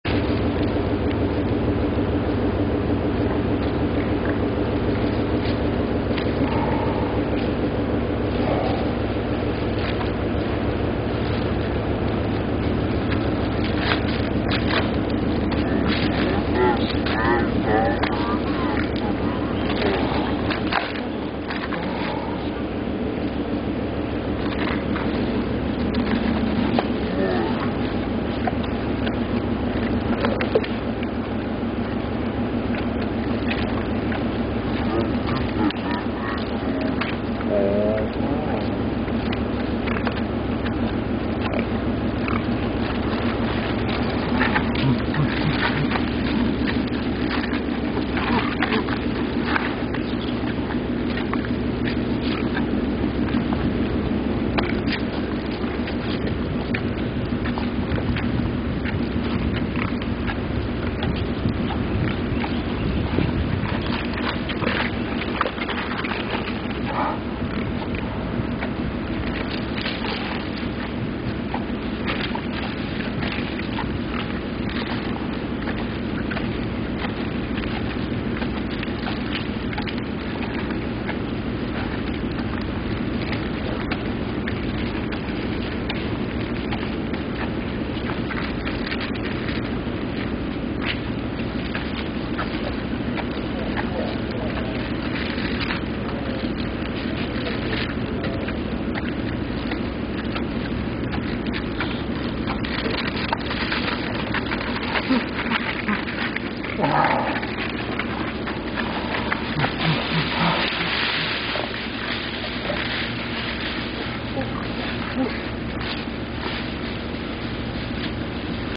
Wild Boars and Piglets eat a Coconut
Free video of wild boars eating coconut. The wild boars are a very cute race of pigs that have stripes on the backs. They are used to humans and this is why the Indian ranger is feeding them with coconuts.

The mother of this pigs is very happy and can be touched, if you offer food for eating to her. Those little baby pigs are very adorable and cute, because they have more stripes than the huge adult boars.

This forest animals are good for the soil in the forest, becasue they fertilize the ground frequently and help to feed the predators, like tigers and wolves. Without those cute, little boars, the tigers will be very endangered, because there would be nothing to eat for them.